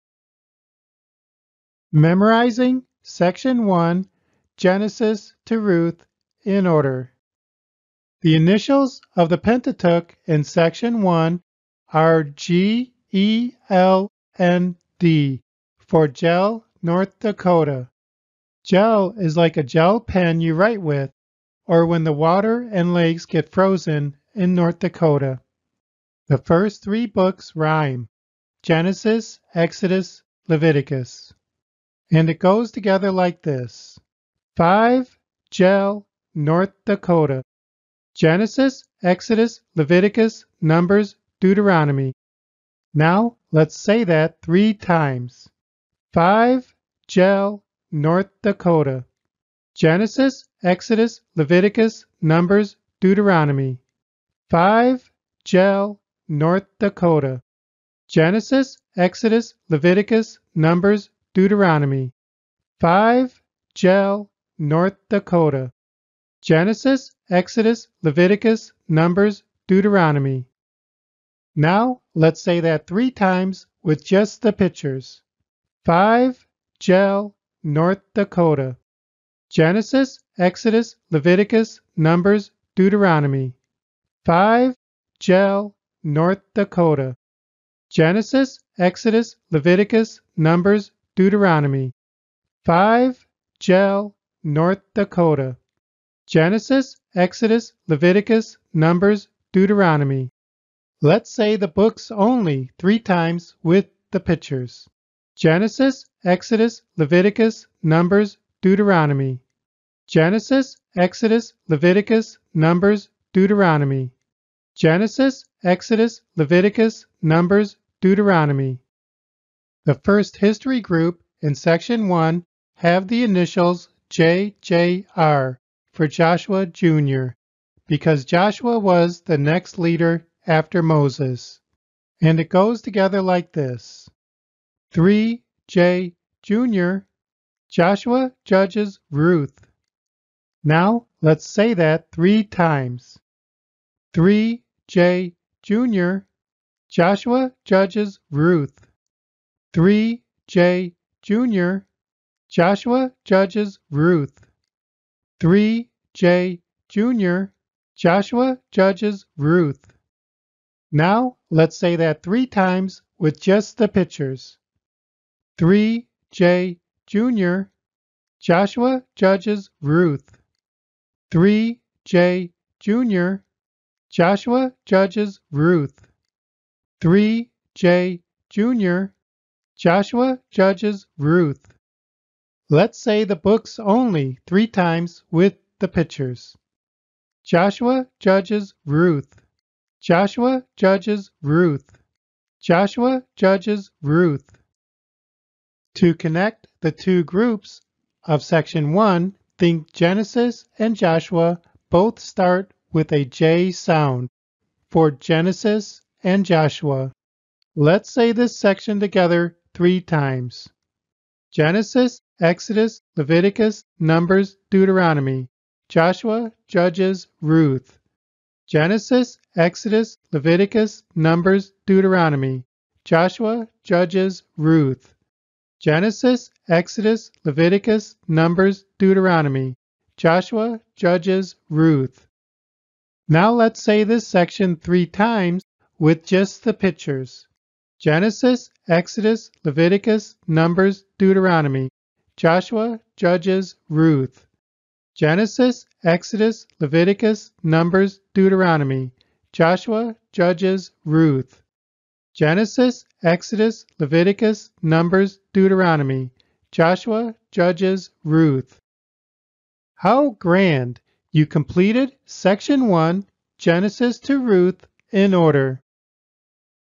Learn B (Section 1)
This video will help you learn the Bible “Books In Order” using the BibleRICH icons.  It will help you understand how the Bible is divided into sections and groups.

By learning and reviewing these sections and groups, you will find the Bible has a logical sequence of sections and groupings which make it easier to memorize the books in order from Genesis to Revelation.